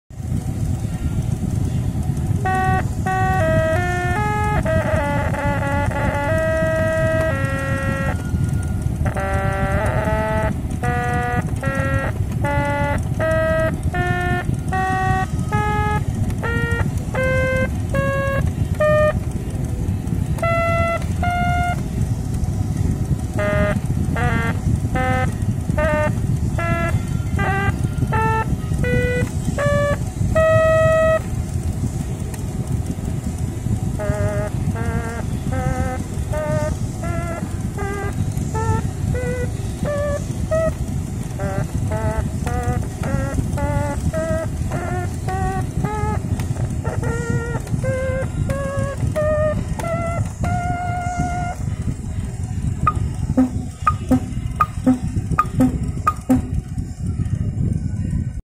Testing Vintage Electronic Organ Super Sound
Supersound organ
- Menggunakan voltase 220v
- Kondisi nyala